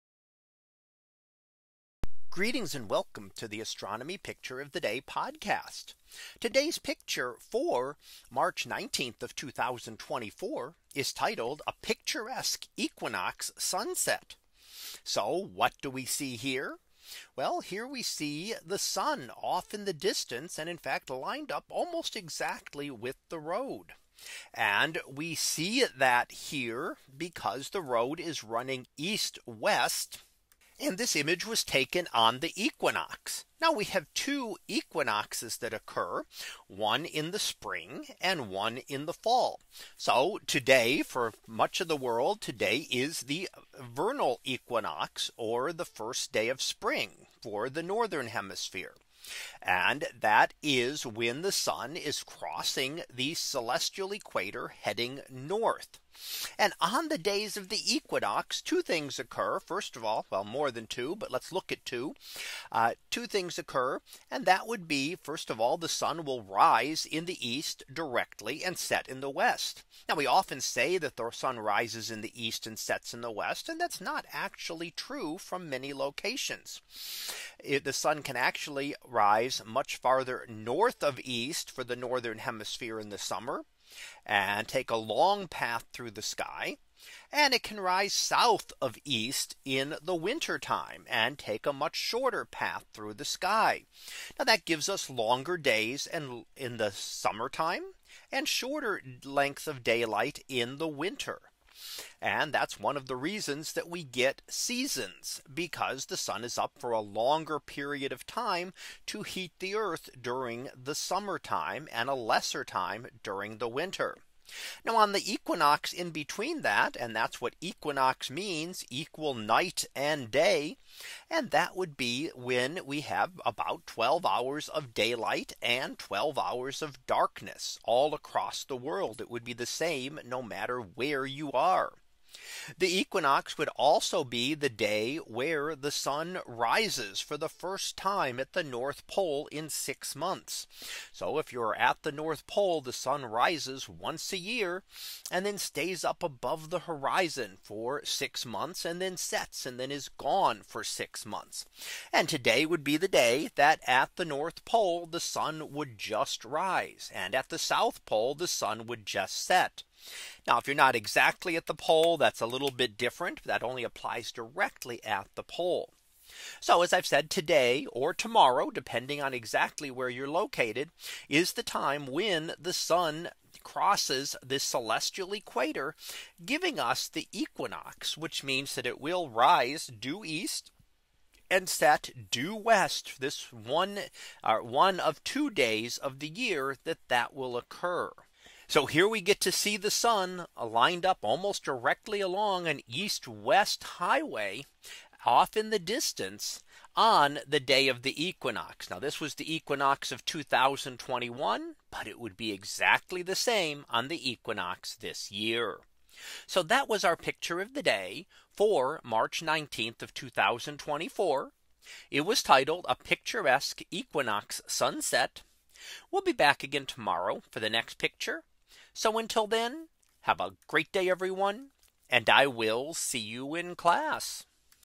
2024 March 19 - A Picturesque Equinox Sunset
In today's image, we see the Sun setting at the end of a road. This road runs roughly east-west so the Sun will appear to rise and set at the end of the road on the two equinoxes that occur each year. For this in the Northern Hemisphere, today is the Vernal Equinox or the first day of spring. On this day, the Sun crosses the celestial equator moving into the northern part of the sky. It is also the day where day and night will be of equal length - 12 hours each.